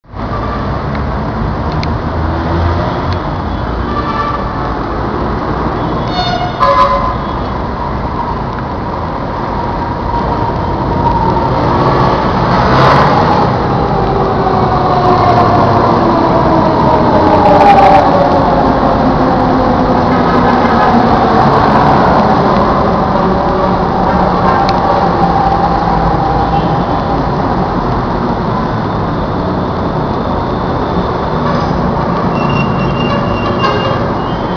LRT1 - 3G LRV at United Nations Station
3G LRV (set 1229 + 1230 + 1231 + 1232)  approaching united nations station